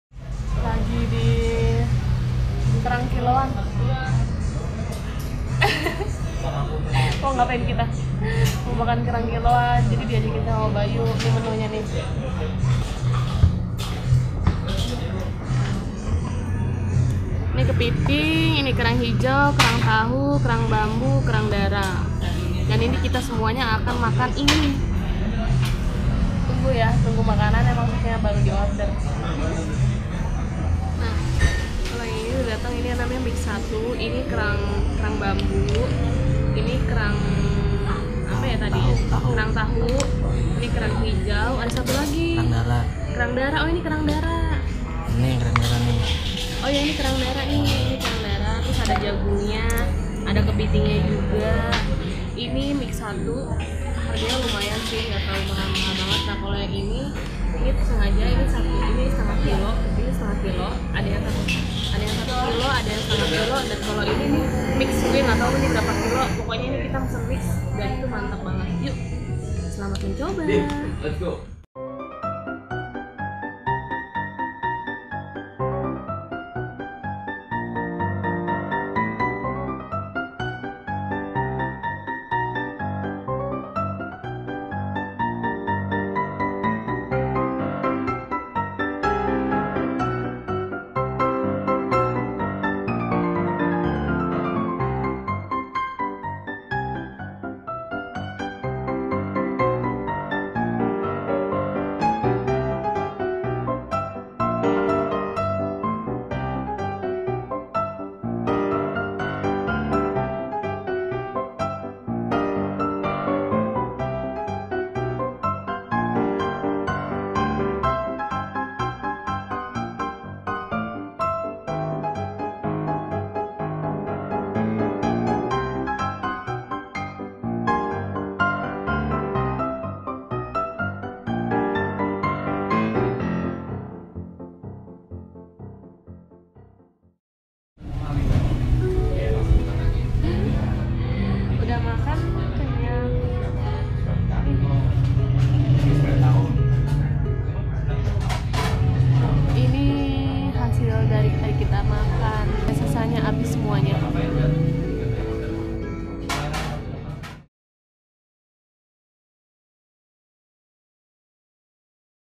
Makan Kerang Kiloan Mengakibatkan Tangan Berdarah I Rbayuaji #15
Berawal dari Penasaran Makan kerang kiloan katanya MAHAL Tapi pas Gw Coba makan Kerang Kiloan Di Daerah Cipete Jakarta Selatan Bersama @annisayudianar MURAH BANGEEET SUMPAH dan ENAK BANGEEET Olahan bumbunya Itu Loh yang Bikin Semangat Gw Makannya Sampe tangan gw BERDARAH !!!